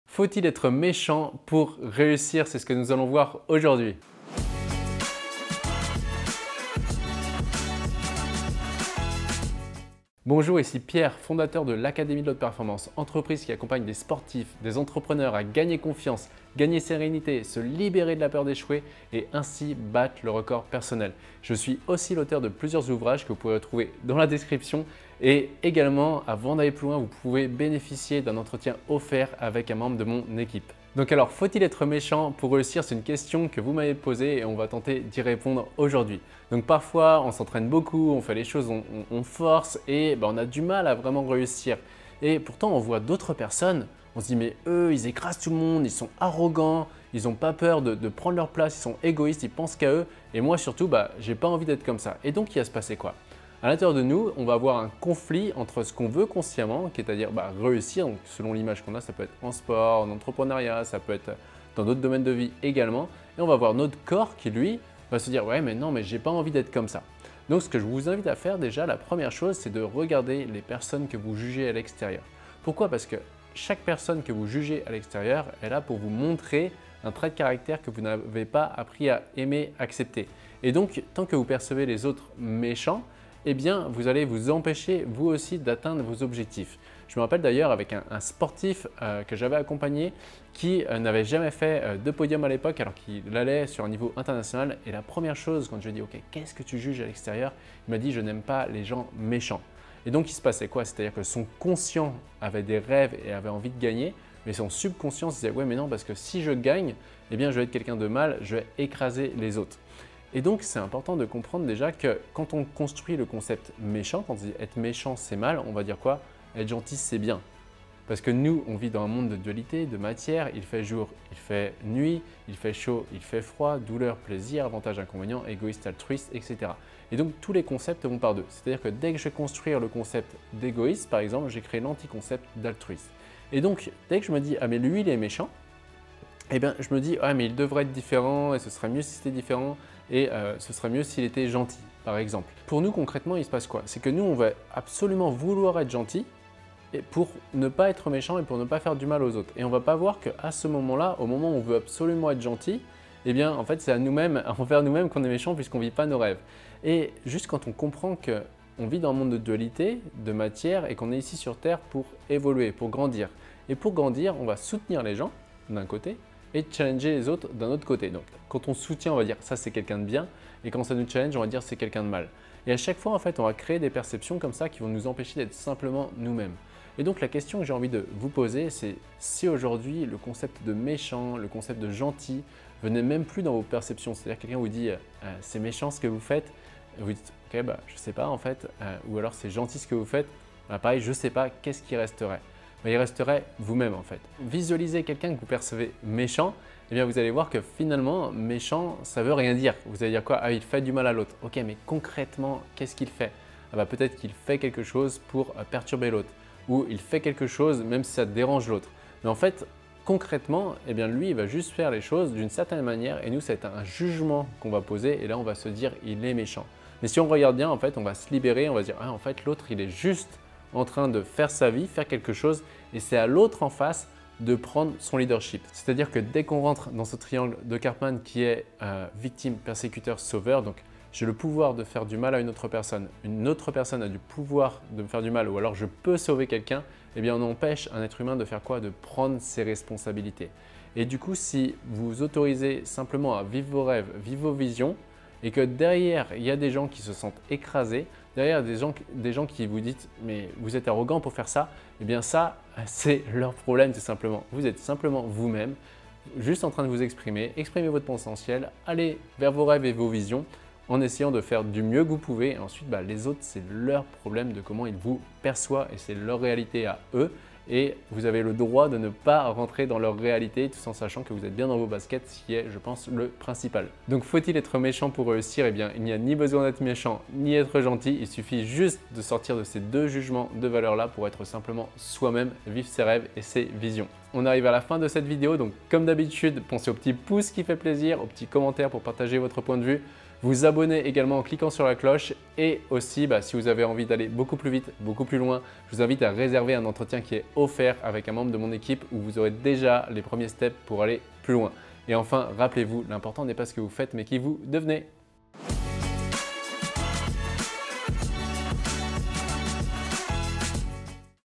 Faut-il être MÉCHANT pour RÉUSSIR ?
Parfois on s'entraîne beaucoup, on force, on a du mal à vraiment réussir !

Et pourtant on voit d'autres personnes, on se dit : "Mais eux ils écrasent tout le monde, ils sont arrogants, ils ont pas peur de prendre leur place, ils sont égoïstes, ils ne pensent qu'à eux..."

Et du coup on en vient à se demander s'il faut vraiment être méchant pour réussir ?

Dans cette vidéo, je vais vous donner les clés pour vous libérer de vos blocages et réussir en étant vous-mêmes.

0:00 Introduction
1:06 Regardez les personnes que vous jugez
1:26 Exemple d'un sportif
1:50 Se libérer des concepts
2:37 Nous sommes sur Terre pour grandir
3:45 Prendre sa responsabilité
4:34 Résumé
4:44 Pour aller plus loin